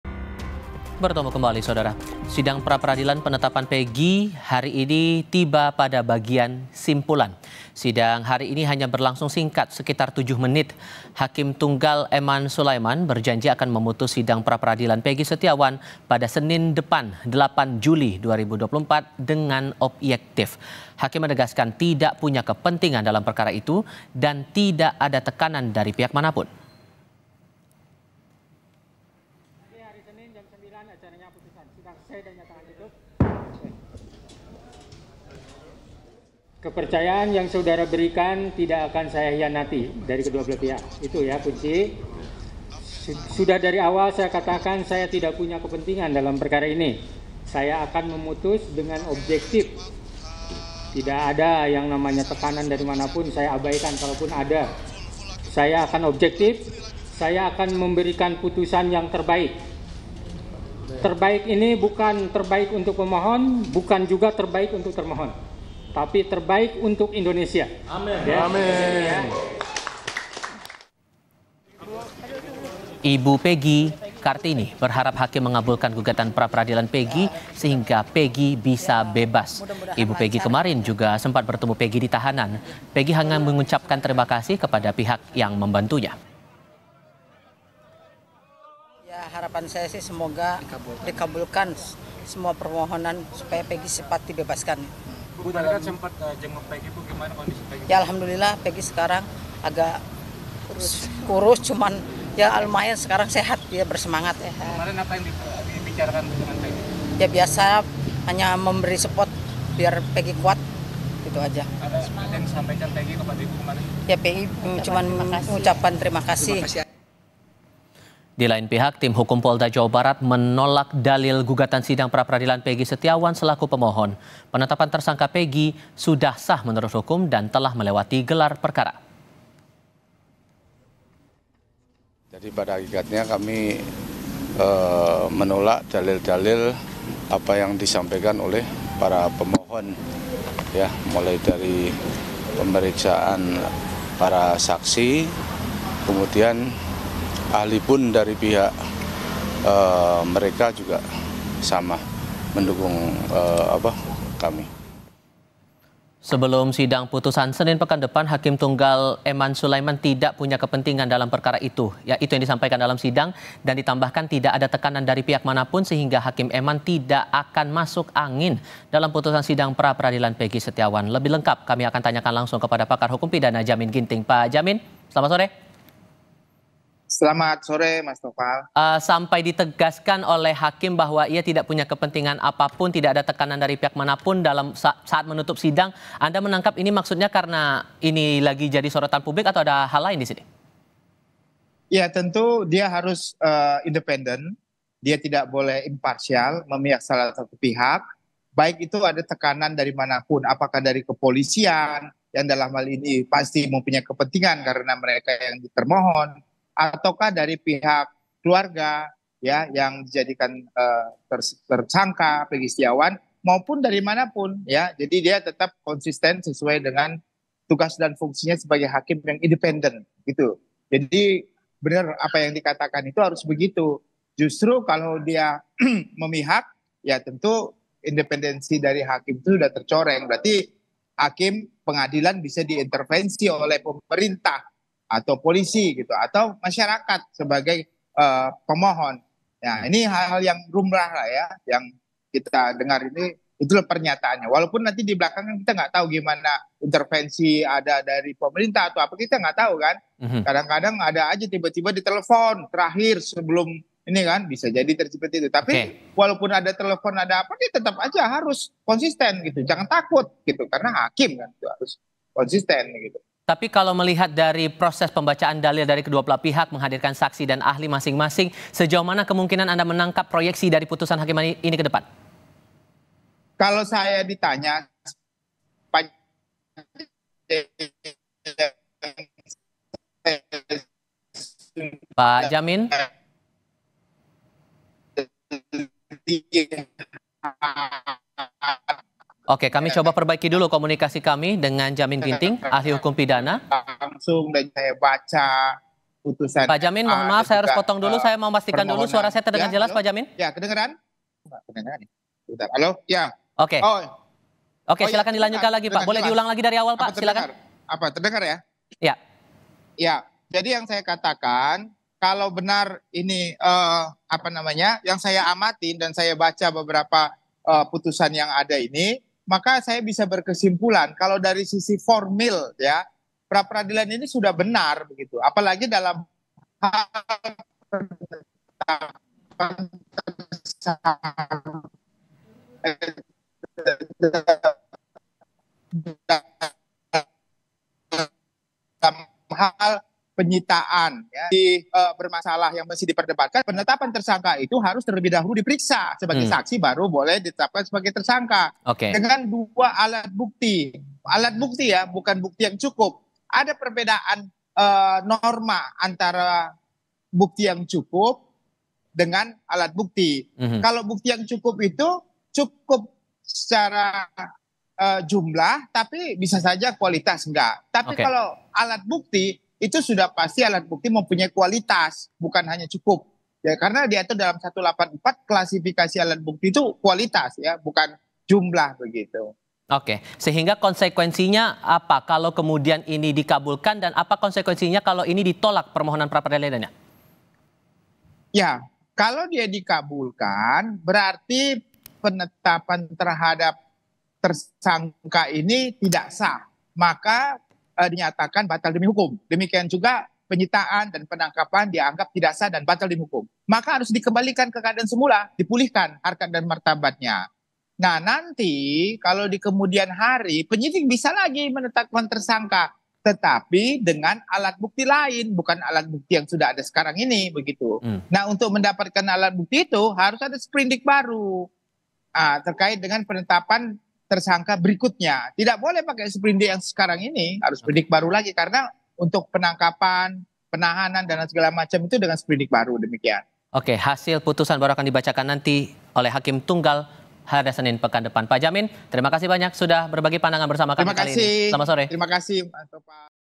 Jelang Putusan Praperadilan Pegi, Hakim Tegaskan Tak Ada Tekanan, Apa Maknanya?
KOMPAS.TV - Jelang Putusan Praperadilan Pegi, Hakim Tegaskan Tak Punya Kepentingan & Tak Ada Tekanan

Kompas TV
Independen | Terpercaya